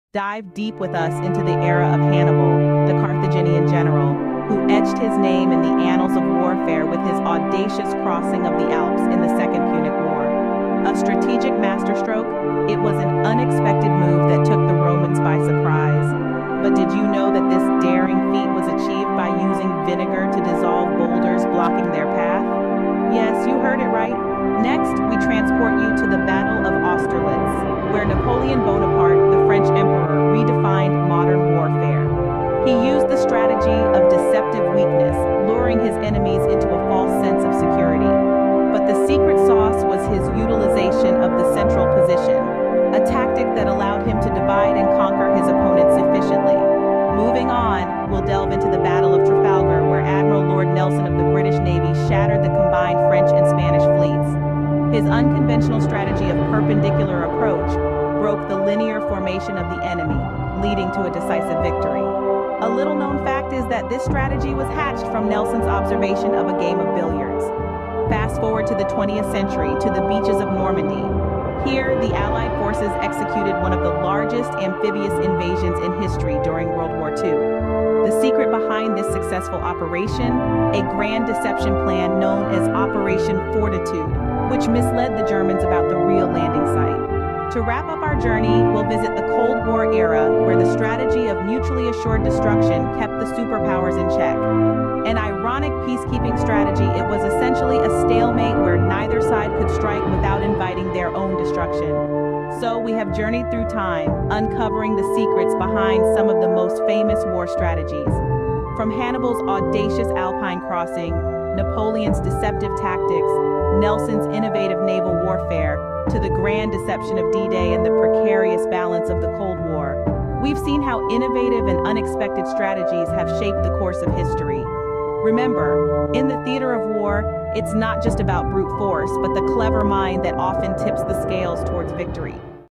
Hidden Tactics & War Games Historical War Strategies Revealed
Discover the intriguing world of historical war strategies with us as we take you on a captivating journey through pivotal moments that have influenced the course of warfare! From the daring Alpine Crossing of Hannibal to the deceptive tactics employed by Napoleon and the innovative naval warfare strategies of Admiral Lord Nelson, witness firsthand how the brilliance of strategic minds has played a crucial role in determining the outcome of battles. Don't miss out on the opportunity to explore these hidden tactics and war games that have shaped history!

Uncover the secrets behind some of history's most famous war strategies in this fascinating video. From Sun Tzu's "The Art of War" to Napoleon's military tactics, learn about the hidden tactics that shaped the outcome of some of the greatest battles in history. Discover how these strategies were developed and executed, and gain insight into the minds of the brilliant military leaders who devised them. Don't miss out on this exclusive look at the tactics that changed the course of history.